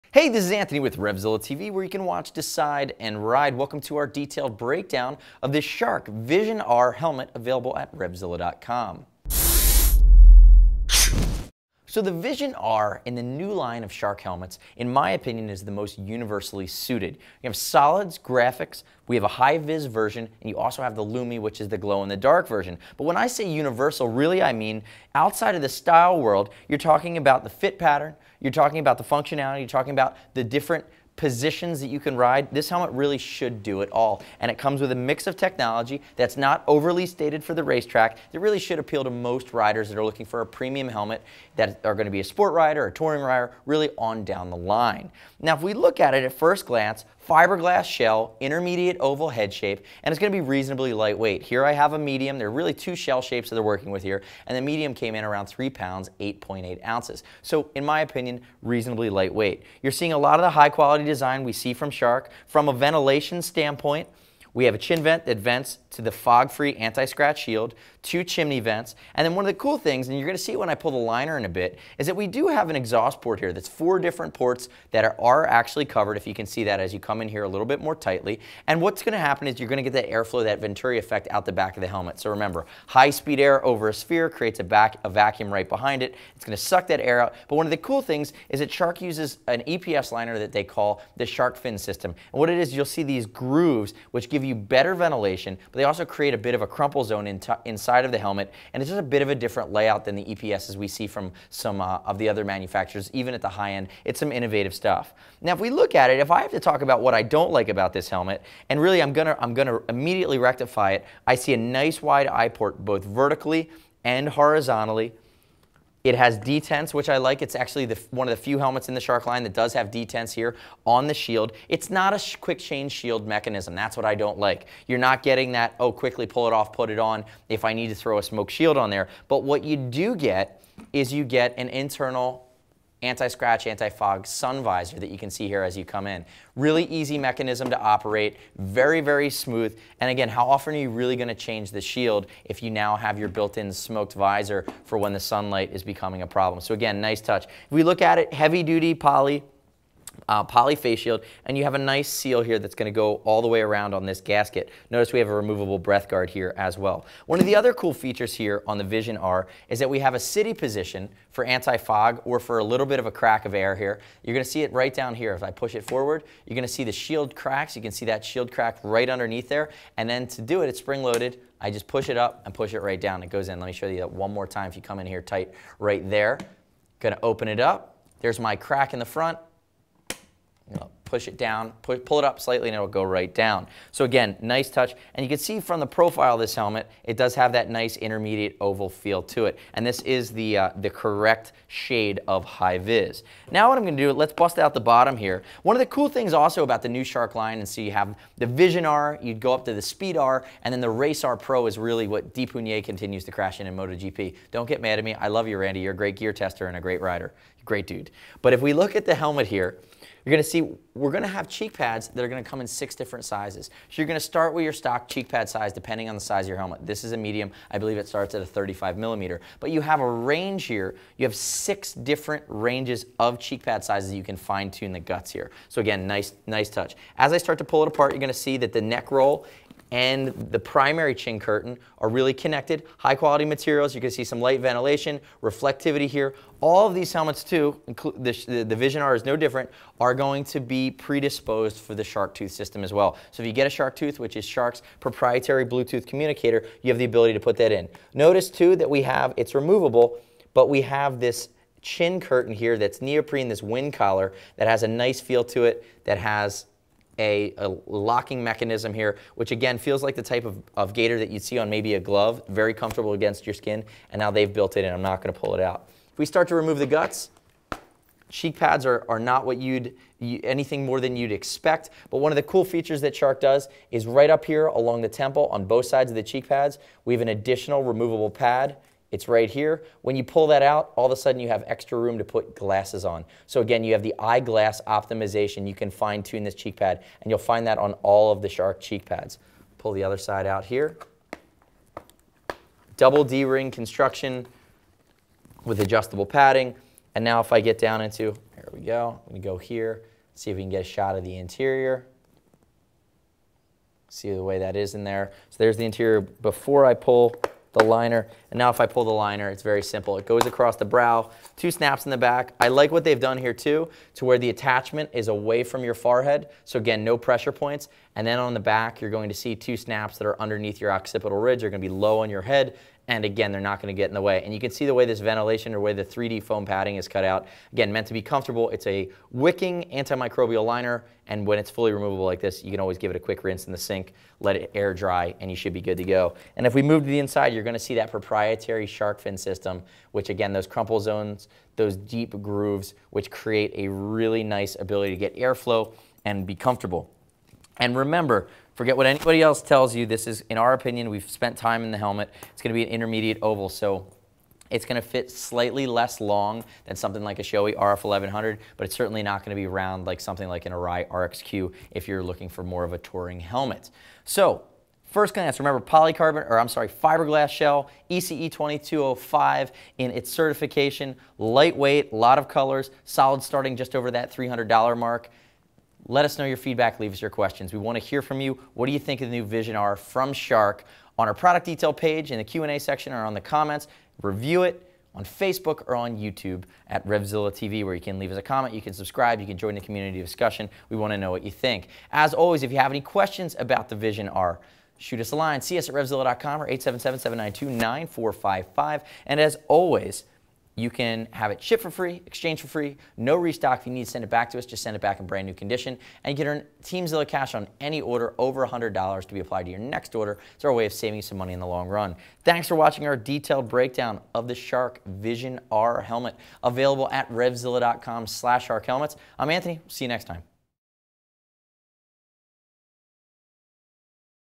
Shark Vision-R Helmet Review at RevZilla.com
Shark Vision-R Helmet Review
 New to the US market, Shark comes on full force with 3 sweet full face helmets, including the Vision-R.  Aimed at the touring and city rider with its extra large eye port means you have a better chance of avoiding obstacles in your path and beyond.  Focusing on safety, the Vision-R uses Shark's "sharkfin" EPS liner that helps channel air when the helmet is on and doubles as a shock absorb-er in a crash.  The shell is a mix of composite fibers arranged in Shark's proprietary "crumple zones" that help absorb and distribute energy in a crash.  The interior is fully removable for cleaning and is prepared to take the Sharktooth BT unit.  The Easy Fit glasses system allows the rider to remove small pads from the cheek pads to better accommodate glasses.  Great venting along with an anti-fog / anti-scratch coating help further enhance the riders vision.  Offered in solids, graphics and even a Glow in the Dark version.